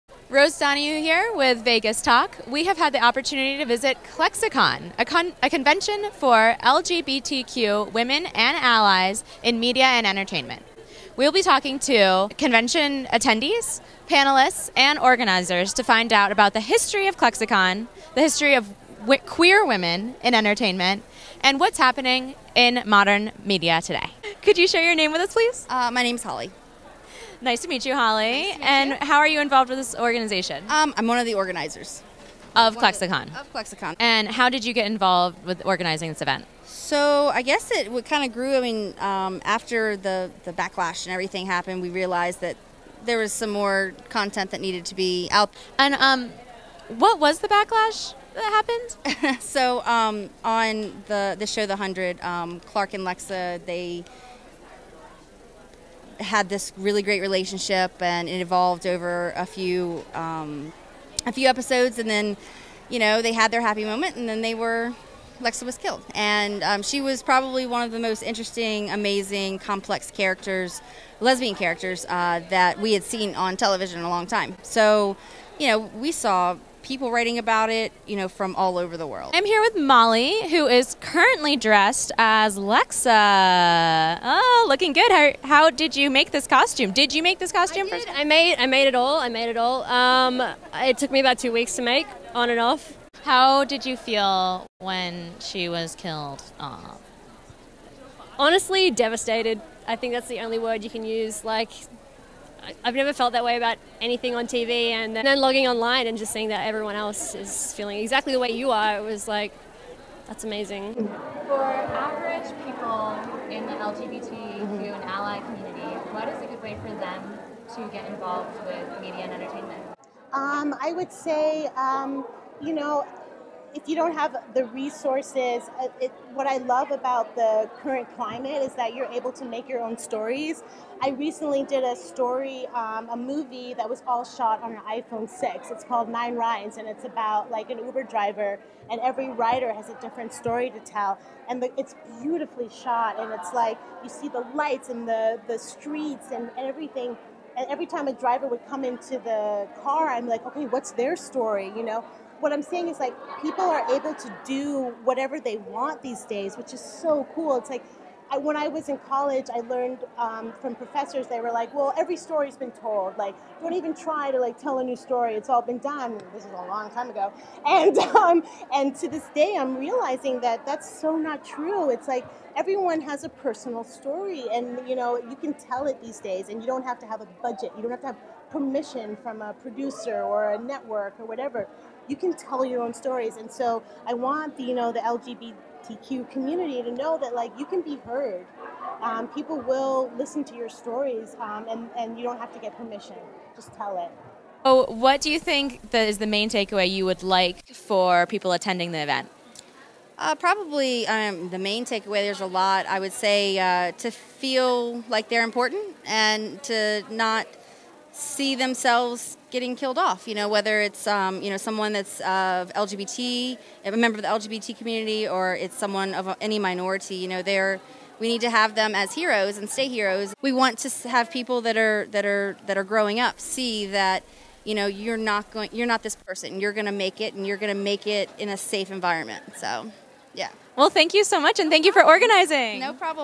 Vegas Talk Goes to ClexaCon
Vegas Talk goes to ClexaCon, a conference for LGBTQ and Allies in Media and Entertainment. Rose finds out about the drama behind what happened to the characters Clarke and Lexa, learns about the experience of fans, and finds out how people can get involved in media and entertainment.